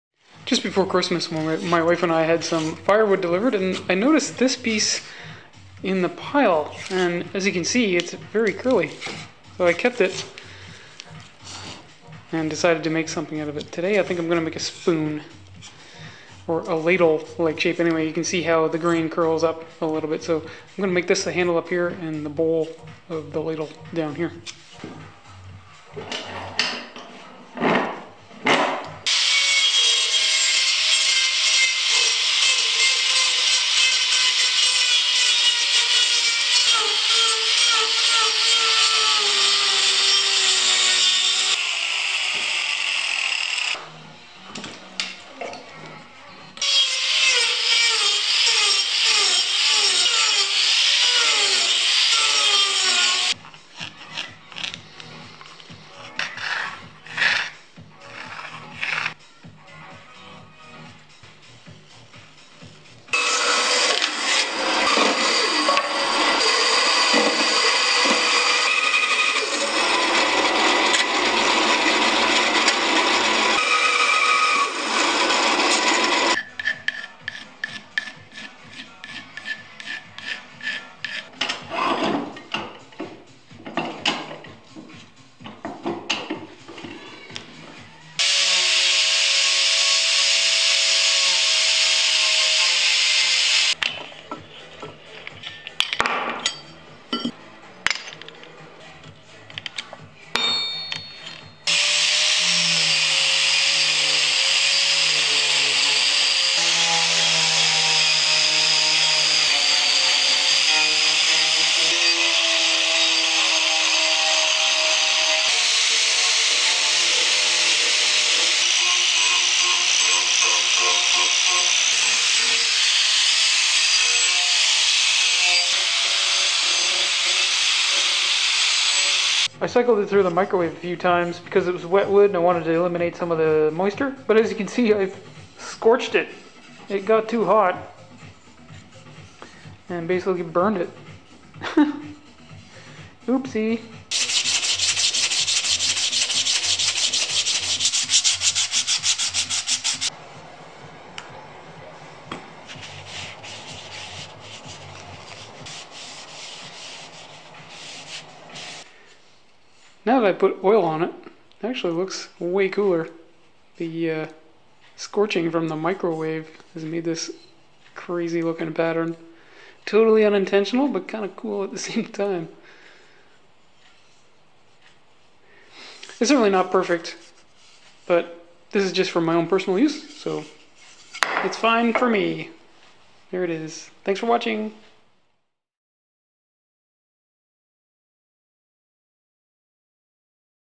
Power Carving a Spoon - Woodworking
I made a wooden spoon from a stick of oak firewood.